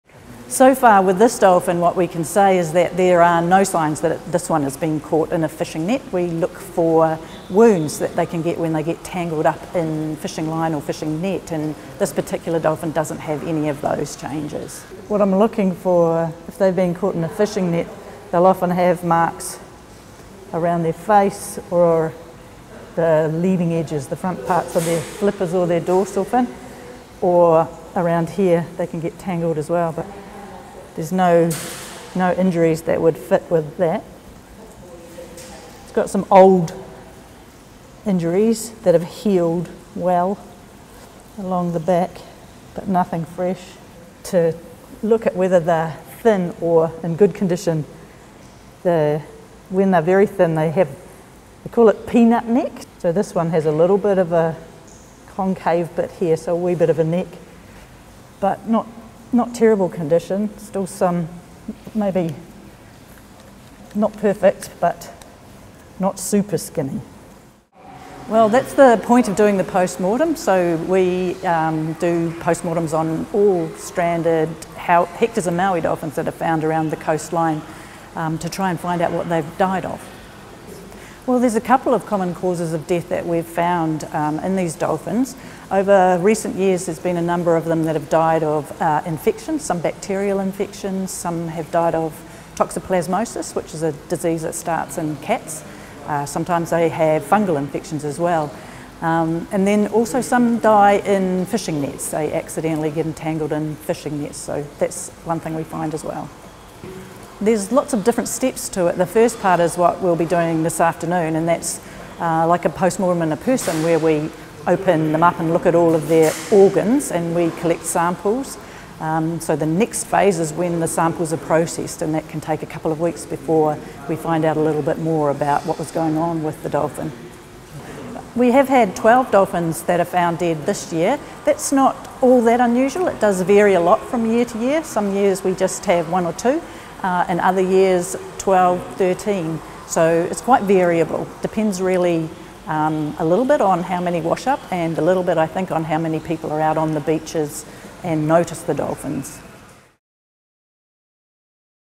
Hector's necroscopy seeks to find cause of death | Massey University
Professor Wendi Roe speaks about the potential cause of death of an endangered Hector's dolphin, found on a Christchurch beach in March 2022.